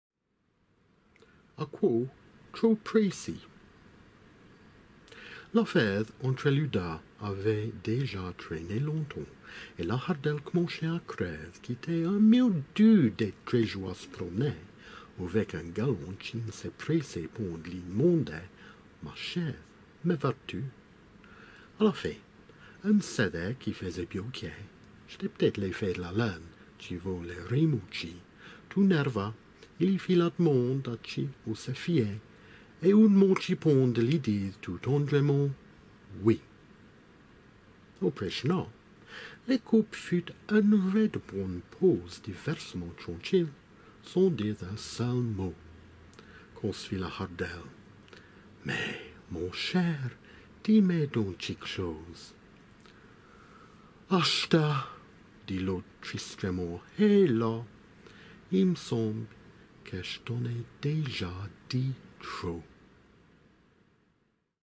Acouo trop prêssi
L'affaire entré lus deux avait dèsjà traîné longtemps,
Et la hardelle c'menchait à craire qu'i l'tait un mio du
D'être tréjous à s'proum'ner avec un galant
Tchi n'sé pressait pon d'l'y d'mander: "Ma chière, mé veurs-tu?"
A la fin, unn' saîrèe qu'i faisait biaû cliai,
- Ch'tait p't'être l'êffet d'la lunne tchi vos lé r'èsmôtchi -
Tout nerveux, i l'y fît la demande à tchi ou sé fiait,
Et ou n'mantchi pon dé l'y dire tout tendrément "Oui."
Après chounna, lé couple furent unn' raide bouanne pôse
Diversément trantchilles, sans dire un seul mot,
Quand s'fît la hardelle - "Mais, man chièr, dis-mé don tchiq'chose:" -
- "Achet'eu," dît l'aûtre tristrement, "hélas, i'm'semble qué j't'en ai dèsjà dit trop!"

J. P.
1916